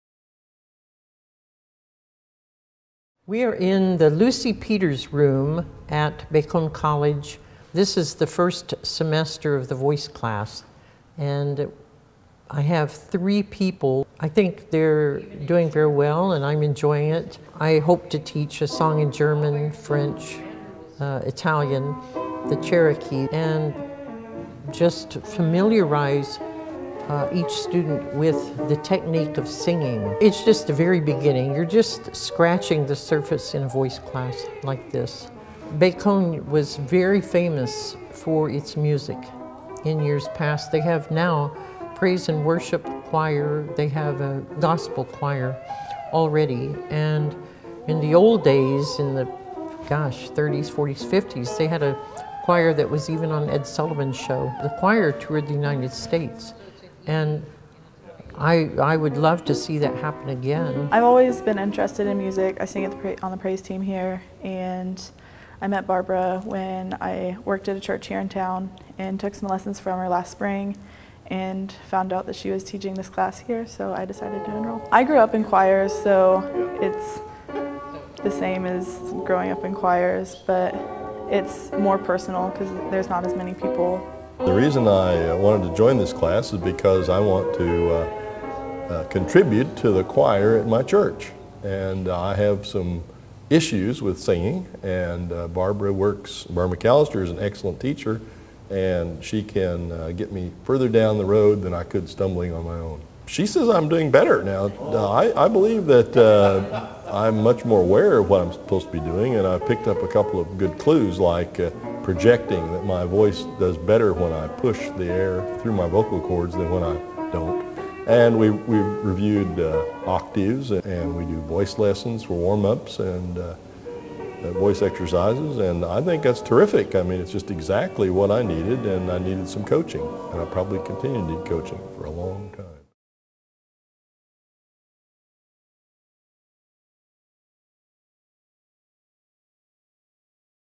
McAlister teaching vocal class at Bacone College
Cherokee Nation citizen Barbara McAlister has an eight-week class in Muskogee, Oklahoma.